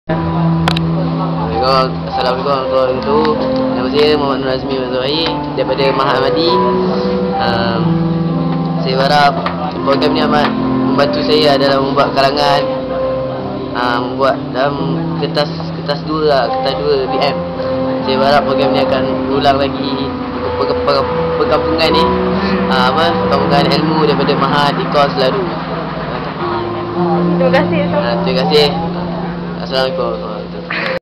Komen peserta Perkampungan Akademik Skor A Bahasa Melayu PMR 2011(AKSES)
komen peserta sepanjang program perkampungan akademik skor A PMR bahasa melayu